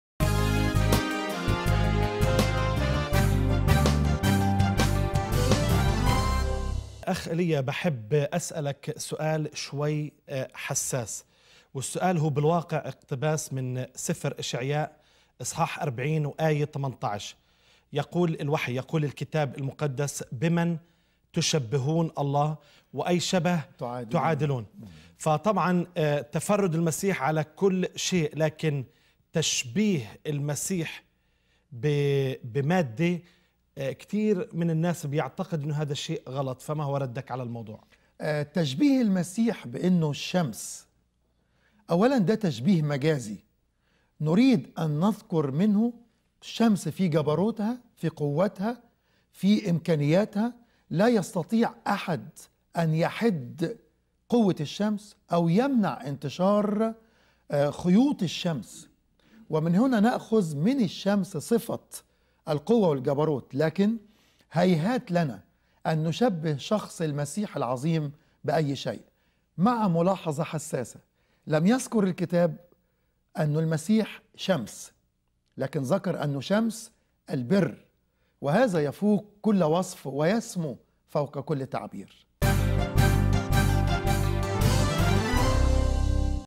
04_760 هل يجوز تشبيه المسيح الرب بأي شيء مادي ؟
يسوع المسيح شمس البر

الحلقة بعنوان يسوع شمس البر

في هذه الحلقة استضاف القس نزار شاهين خادم الرب القس إيليا موريس ليشرح لماذا قيل عن المسيح أنه شمس البر والشفاء في أجنحتها

يمكنك أيضاً متابعة كل جديد في خدمتنا والتفاعل مع أهم مقاطع الفيديو التعليمية من خلال متابعة صفحتنا الرسمية على الفيس :